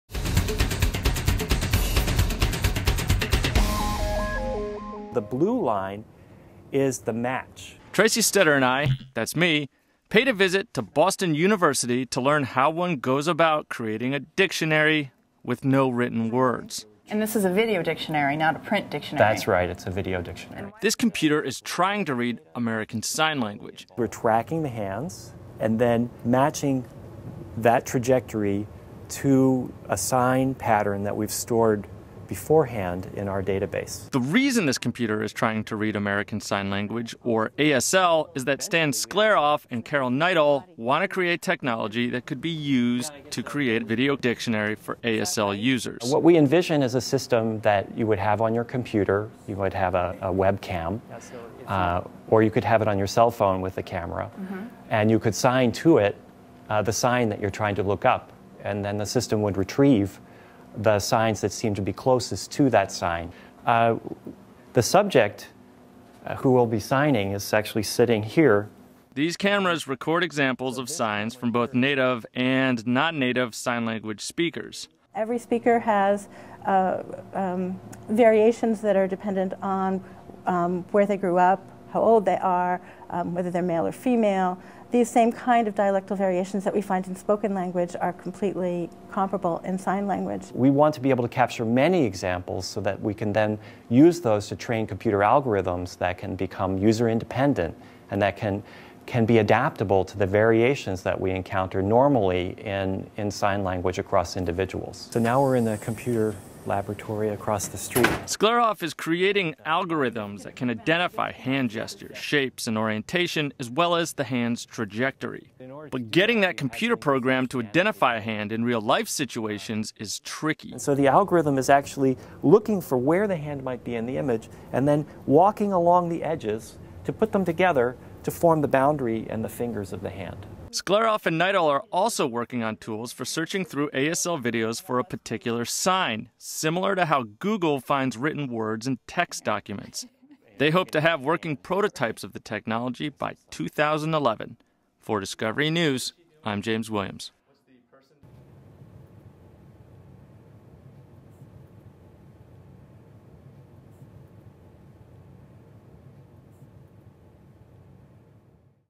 Computers That Can Read Sign Language
Discovery-News.com: Getting a computer to recognize sign language is tough, but researchers at Boston University are getting a hold on the solution.